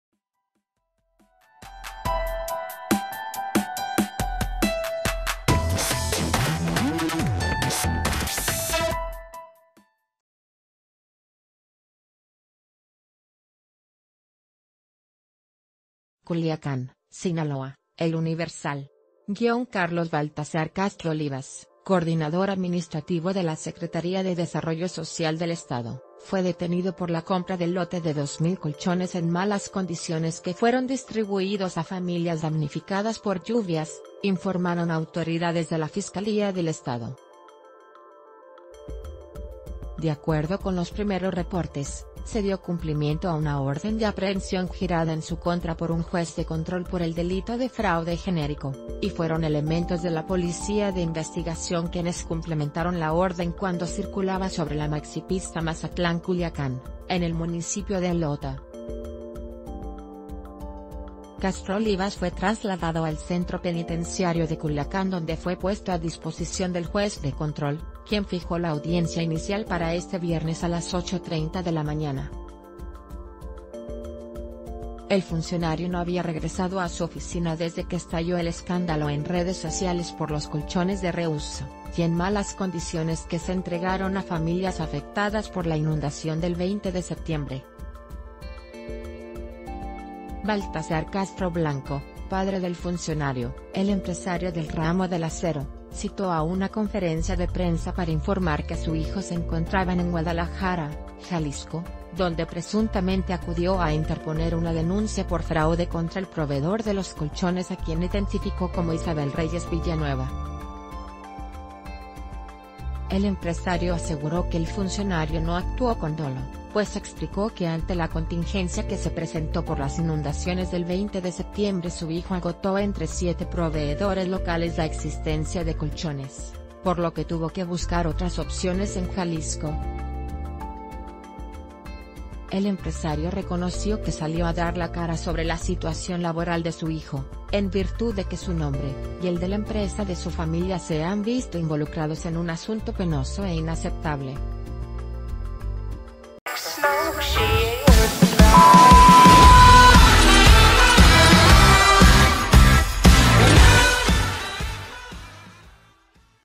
Funcionario que compró colchones reciclados es detenido - El Diario de Yucatán
CULIACÁN, Sinaloa (El Universal).- Carlos Baltazar Castro Olivas, Coordinador Administrativo de la Secretaría de Desarrollo Social del Estado,  fue detenido por la compra del lote de 2 mil colchones en malas condiciones que fueron distribuidos a familias damnificadas por lluvias, informaron autorida...